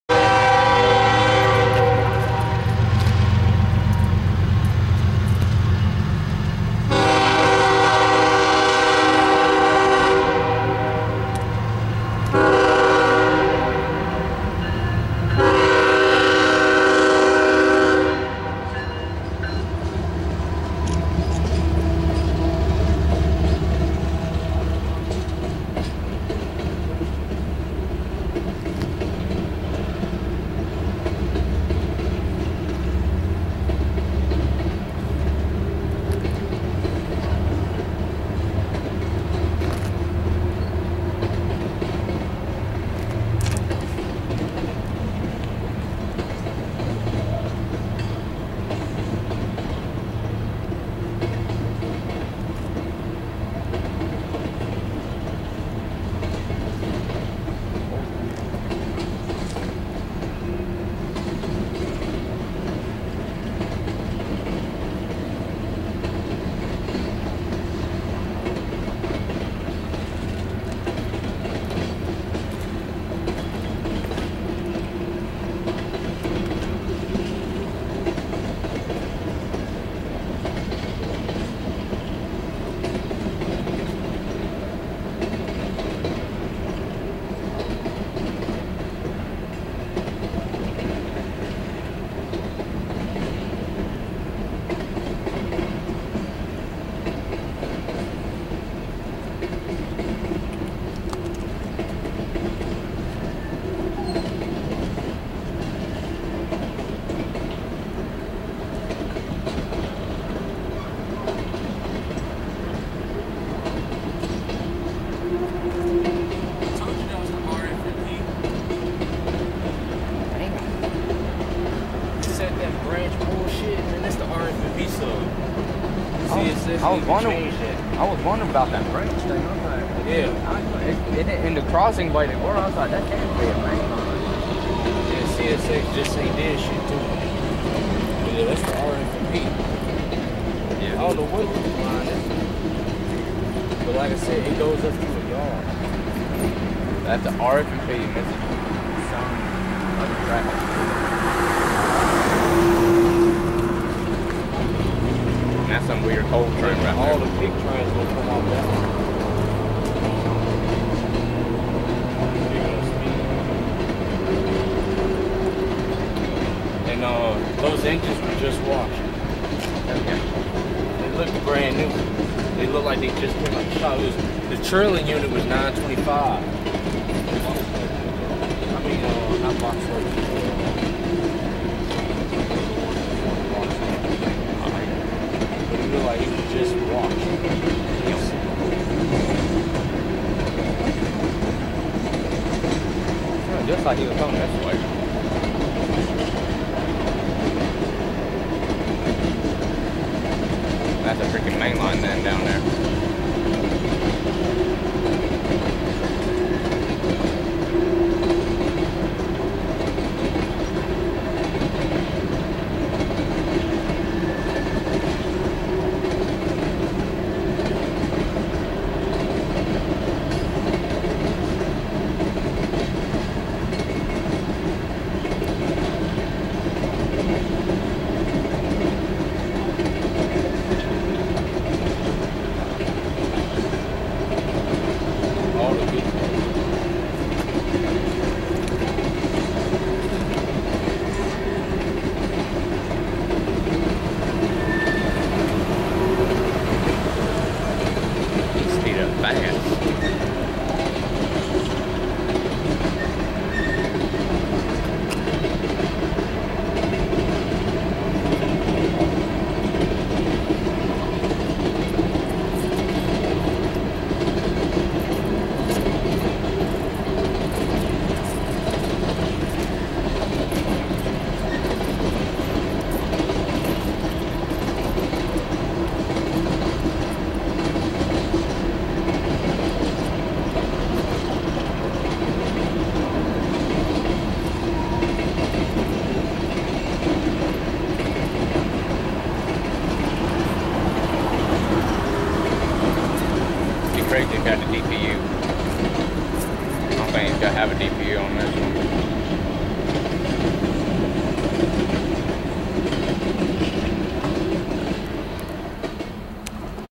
CSX 833 leads CSX Empty Northbound Coal Train in Richmond, VA
CSX RF&P Sub.
Milepost CA 85.7
4/10/13
2:00a.m.
NB CSX Empty Coal Train rolls north in Richmond, VA with a clean pair of Gevos leading. Please Enjoy!
CSX: Coal Trai
CSX: 833(ES44AH)
CSX: 925(ES44AH)
Northbound
Coal Train